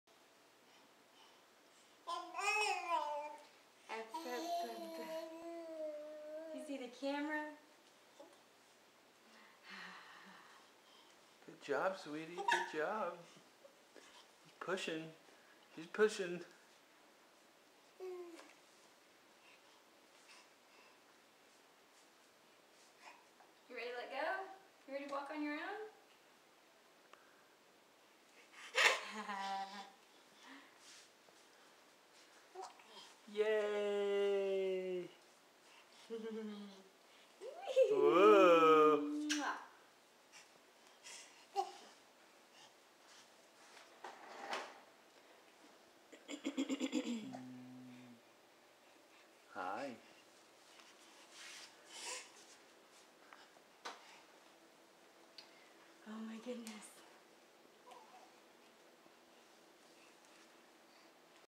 Walking With Help
Alexis is beginning to take steps, with help.  It will probably be a while before any REAL walking, but this isn't bad training.